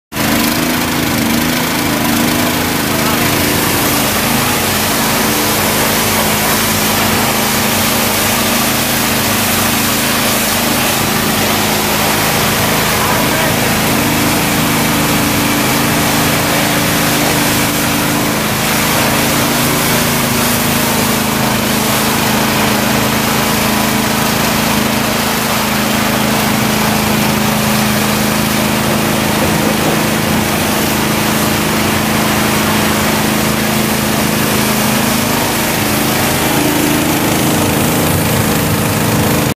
2017 Magnum 4000 pressure washer for sale at auction | bidding closes April 10, 2019
2017 Magnum 4000 pressure washer
Single cylinder gas engine
15 HP
Diesel fired burner
Electric start
Hose
Wand